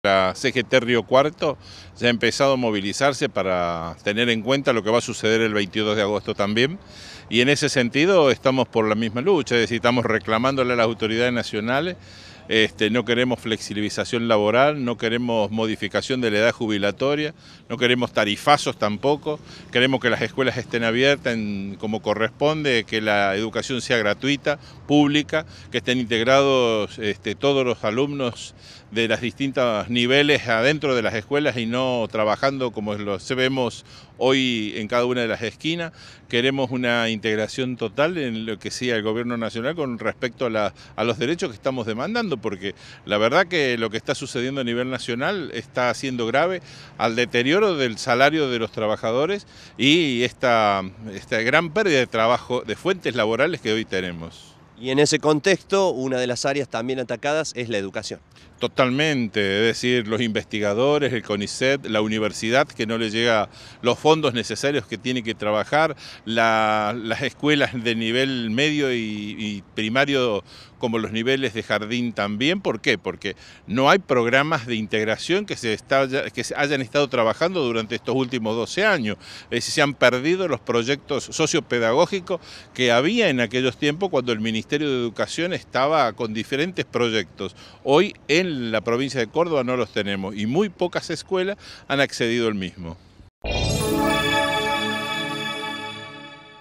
RECLAMAN PAN, TECHO Y TRABAJO DIGNO - JUAN SIMES
MOVILIZACION DE TRABAJADORES CONTRA LA POLITICA NACIONAL
Facebook.com/uniriotv
Twitter.com/uniriotv